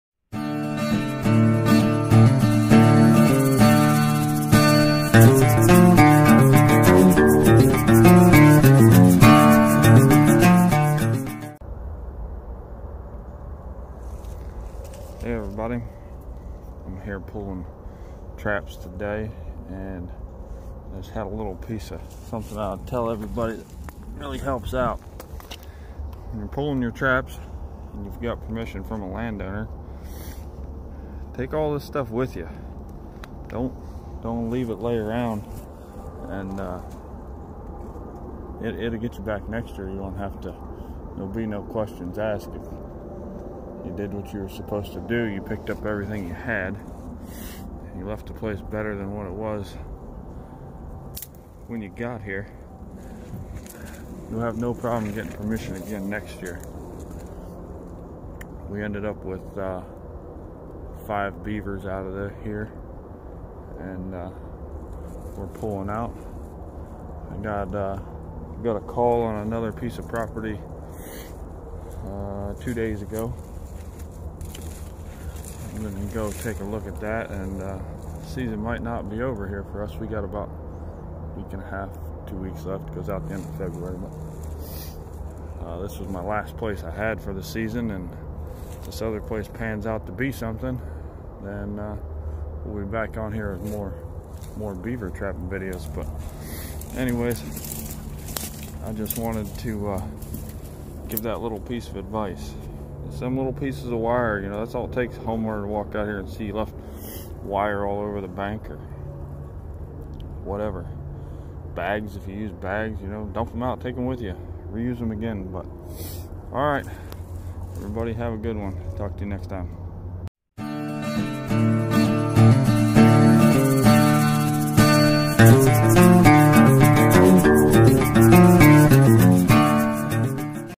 Trapping advice
Quick tip on maintaining permission. And update on beavers.